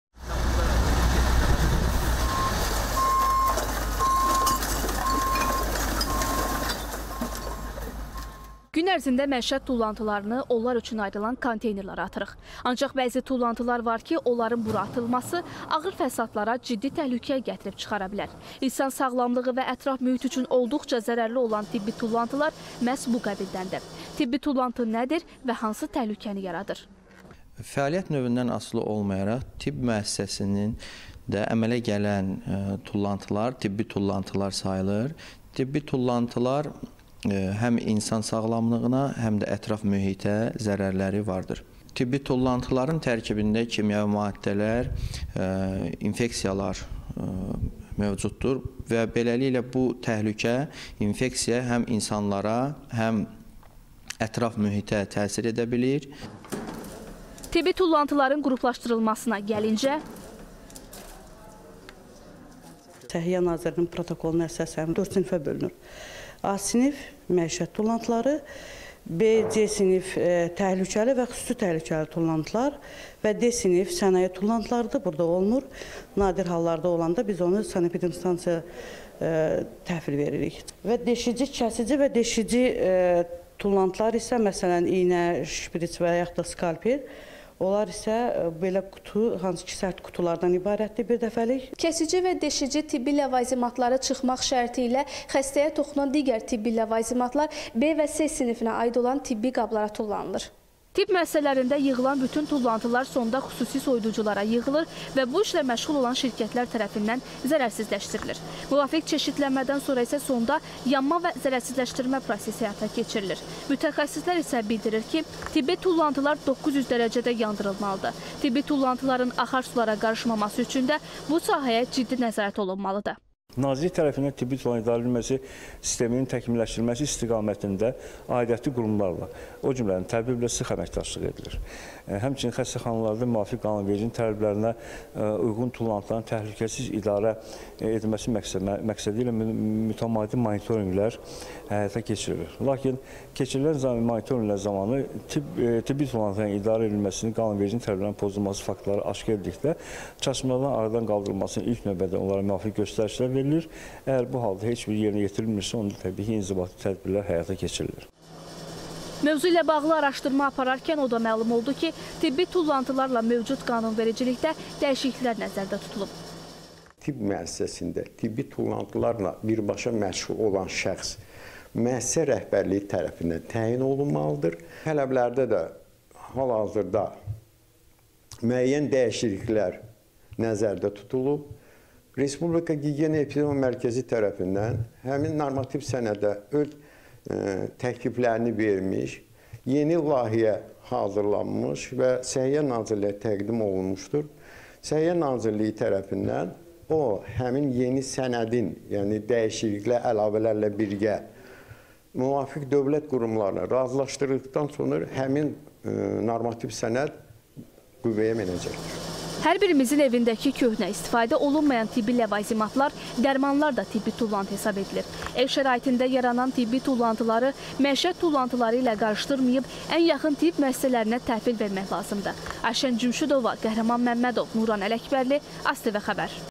Tibbi tullantılar ətraf mühitə hansı təhlükələr yaradır? - “Kimyəvi maddələr, infeksiyalar var”
TİBBİ TULLANTILARLA BAĞLI TƏHLÜKƏ